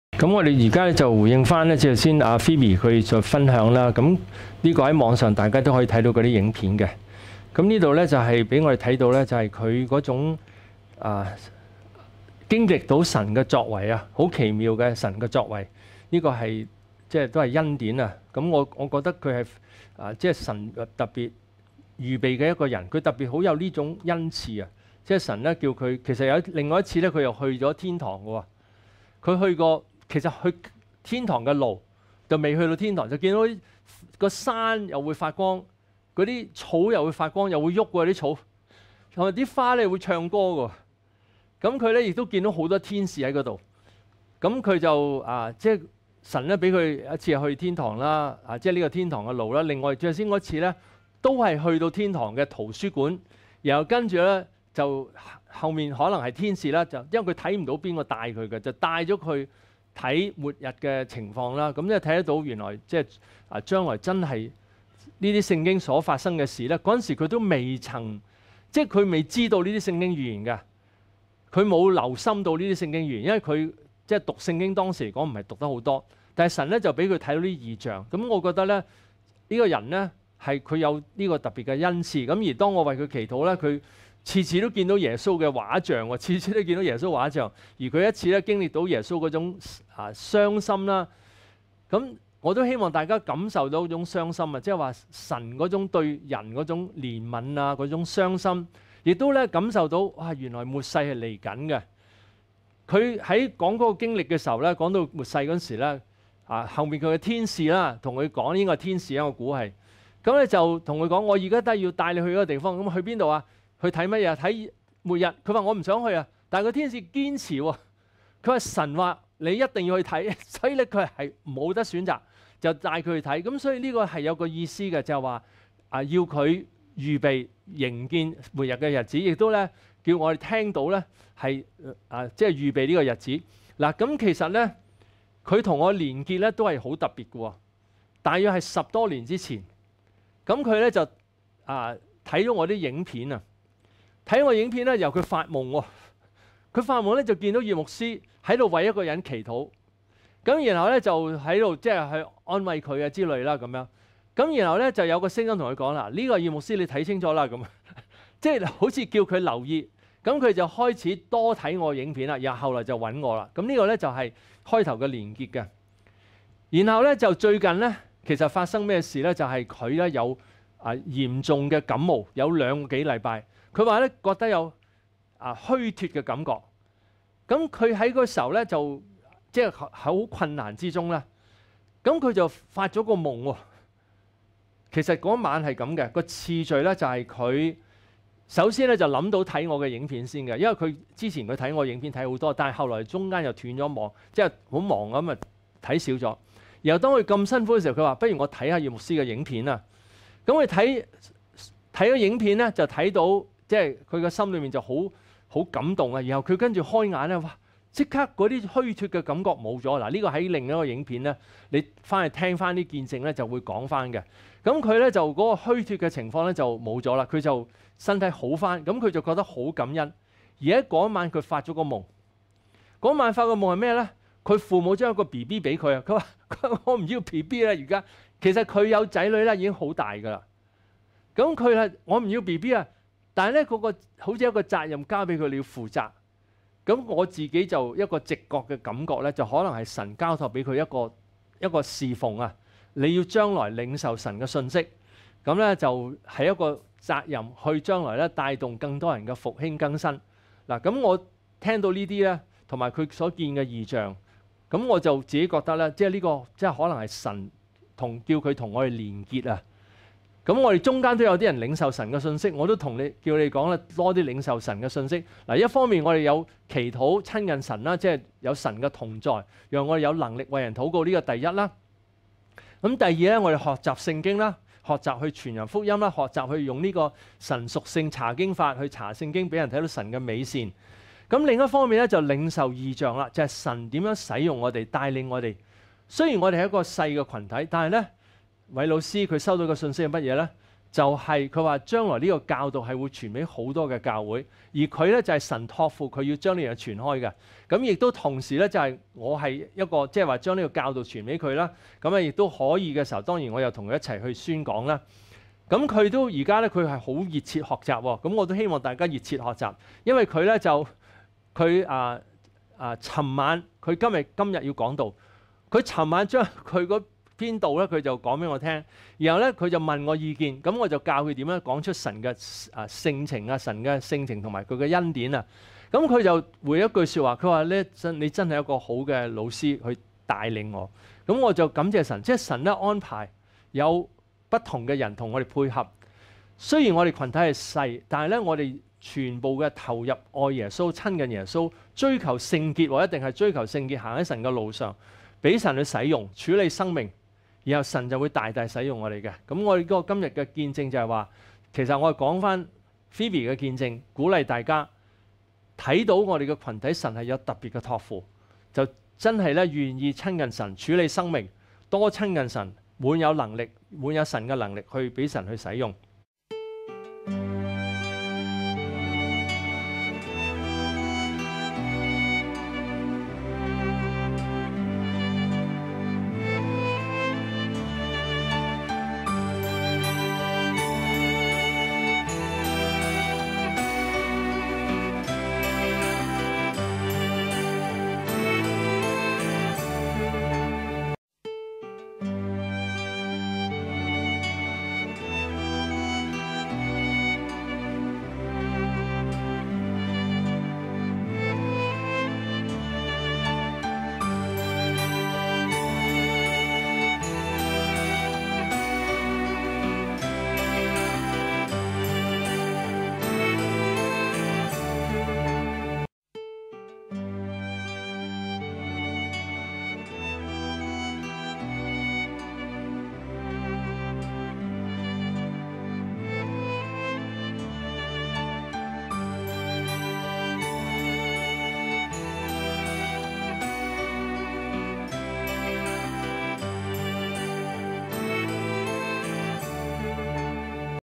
葉牧師回應關於末日的異象 - 葉牧師 2024.06.30
葉牧師回應關於末日的異象

 1. 與主的關係：怎樣建立與主相愛的關係，怎樣從主的愛得力，怎樣深信神的供應和計劃。
 2. 處理生命：怎樣不被人、罪、負面思想情緒和潛意識、婚姻和生活的問題、和侍奉的問題影響。
3. 進入不同層次的侍奉：侍奉可以由簡單的生活見證，到建立別人的靈命，或進入策略性的侍奉。